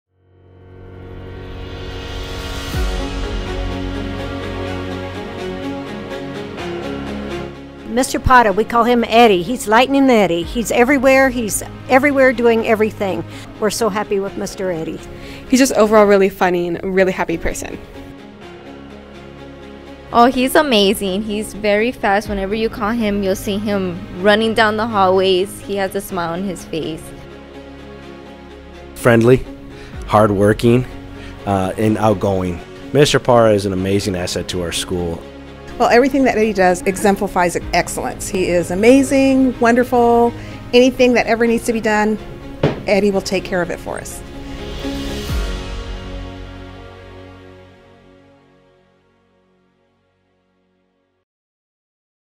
Eduardo Parra Charles MS
Congratulations Eduardo Parra of Charles Middle School. El Paso ISD 2023-24 educational support personnel of the year finalist.